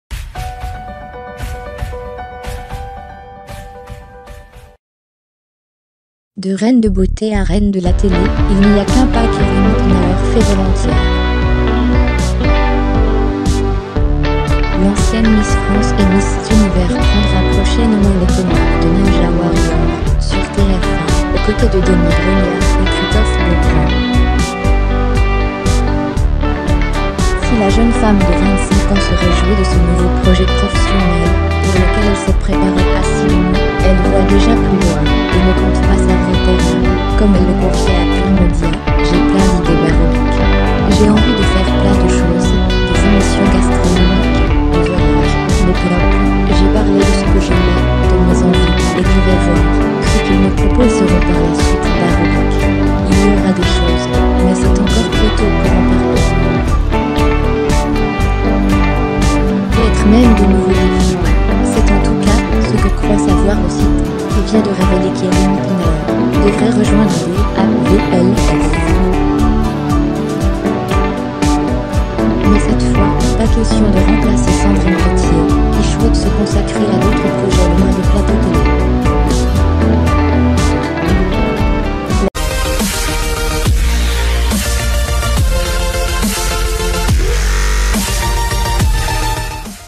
Iris Mitte­naere rejoint Danse avec les stars mais dans quel rôle ? - Gala
De reine de beauté à reine de la télé, il n’y a qu’un pas qu’I­ris Mitte­naere fait volon­tiers. L’an­cienne Miss France et Miss Univers pren­dra prochai­ne­ment les commandes de Ninja Warrior, sur TF1, aux côtés de Denis Brogniart et Chris­tophe Beau­grand. Si la jeune femme de 25 ans se réjouit de ce nouveau projet profes­sion­nel, pour lequel elle s’est prépa­rée assi­dû­ment, elle voit déjà plus loin et ne compte pas s’ar­rê­ter là comme elle le confiait à Pure­me­dias “J'ai plein d'idées ! J'ai envie de faire plein de choses, des émis­sions gastro­no­miques, du voyage, les people… J'ai parlé de ce que j'aimais, de mes envies, et je vais voir ce qu'ils me propo­se­ront par la suite ! Il y aura des choses, mais c'est encore trop tôt pour en parler”. Peut-être même de nouveaux défis. C’est en tout cas ce que croit savoir le site, qui vient de révé­ler qu’I­ris Mitte­naere devrait rejoindre DAVLS. Mais cette fois, pas ques­tion de rempla­cer Sandrine Quétier, qui shouaite se consa­crer à d'autres projets loin des plateaux télé. La jolie brune devrait en effet fouler le parquet de l’émis­sion en tant que candi­date. D’ailleurs, elle s’ap­prê­te­rait à passer des audi­tions. Une propo­si­tion qu’elle avait pour­tant refusé à deux reprises avant de deve­nir le nouveau visage de TF1, comme le rappelle Pure­me­dias. Nul doute que la chaîne mise tout sur cette nouvelle recrue pour appor­ter une touche de glamour à ce show qui dure depuis main­te­nant six ans. Sans comp­ter que son déhan­ché devrait redy­na­mi­ser les audiences. Crédits photos :  RACHID BELLAK / BESTIMAGE